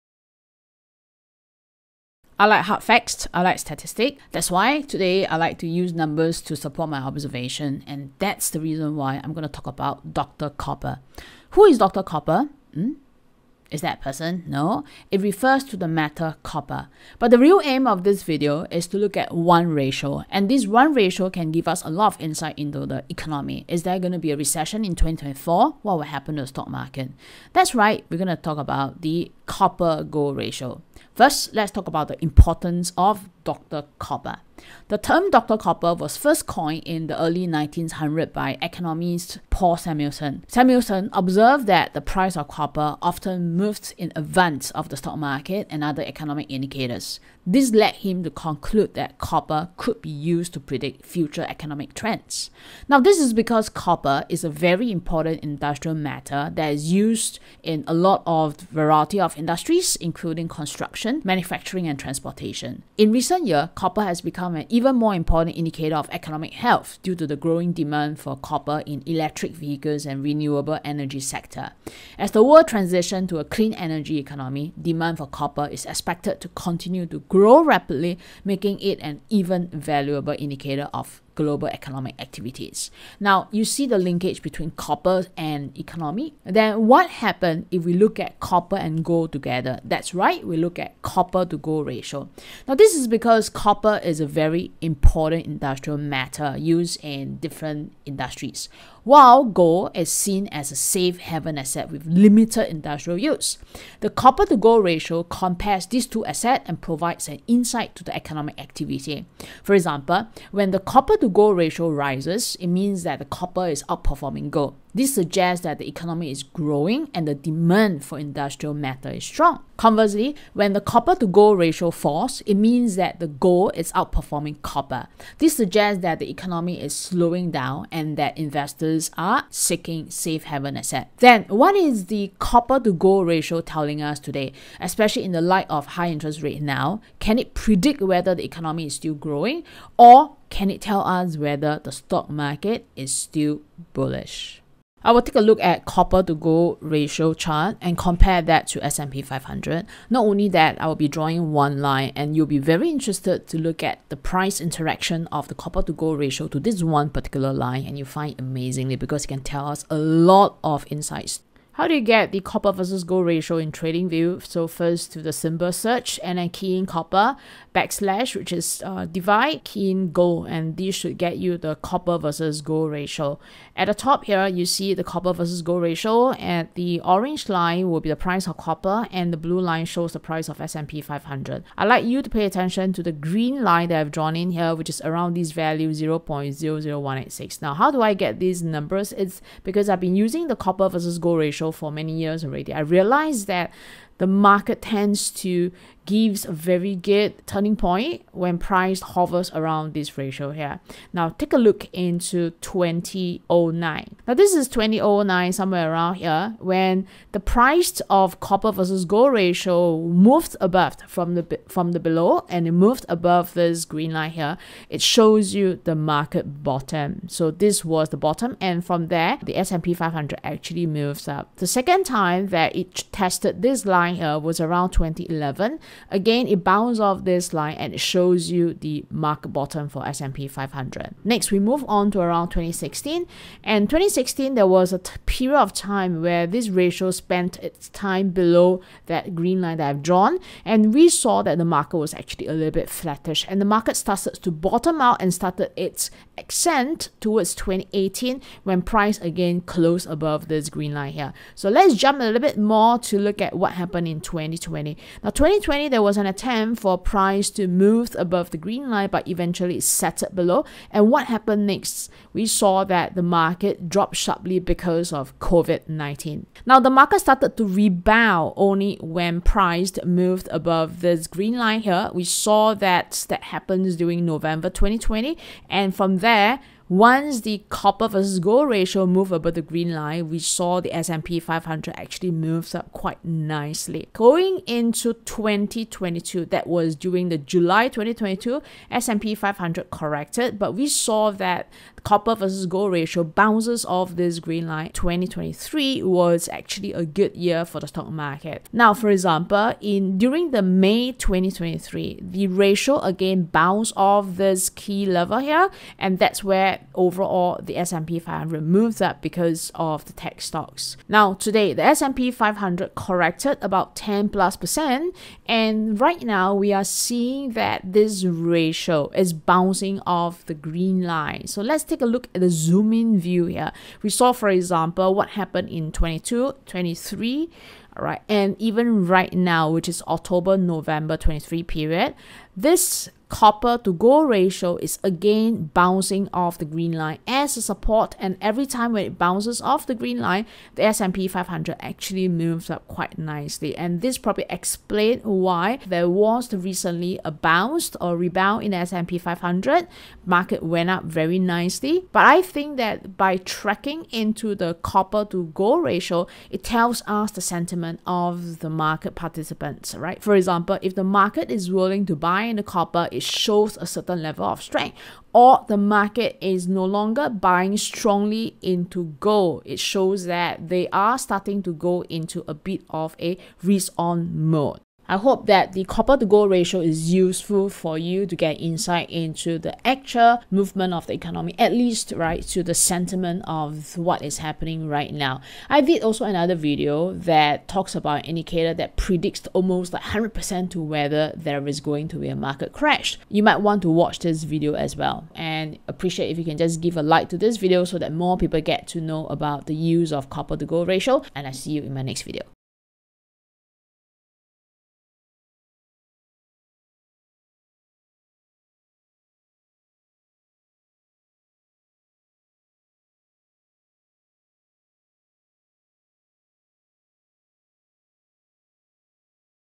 Copper to Gold Ratio: A 100% Accurate Predictor of Market Bullishness
In this video, we take a deep dive into the copper to gold ratio and its uncanny ability to predict stock market movements. As copper is a key industrial metal, its price is closely tied to the health of the global economy. On the other hand, gold is often seen as a safe haven asset, sought after during times of uncertainty. By analyzing the historical relationship between these two metals, we can gain valuable insights into the current state of the market and make informed investment decisions.

00:00 Introduction
00:31 Importance of Dr. Copper
01:27 What happens if we look at copper and gold together?
02:41 Copper-Gold Chart
06:14 Ending remarks